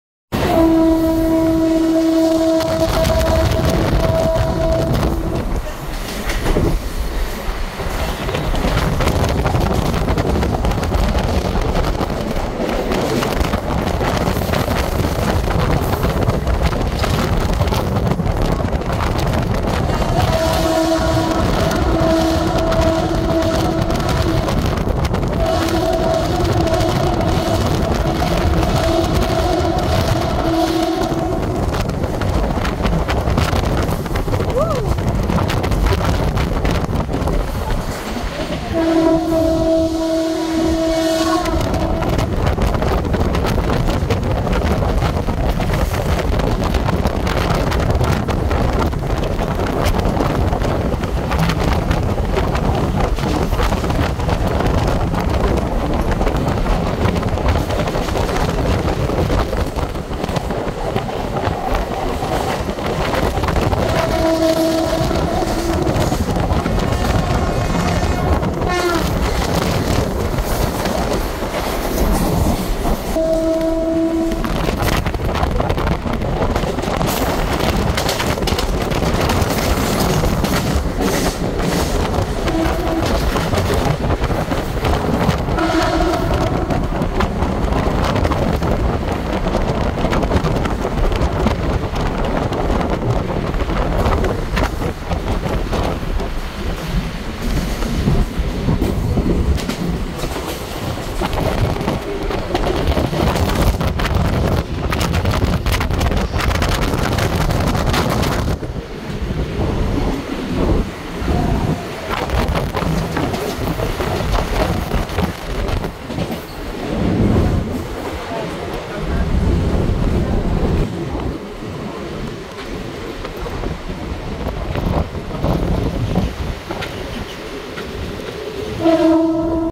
Rampage.. MPS Blast Past Bandra
This One of its Kind, DDR-VR Fast service halts only at Andheri Dahisar Bhayandar and Vasai and takes all by surprise with its Out-of-my-Way approach.

Here's one such.. as it rockets past Bandra at MPS.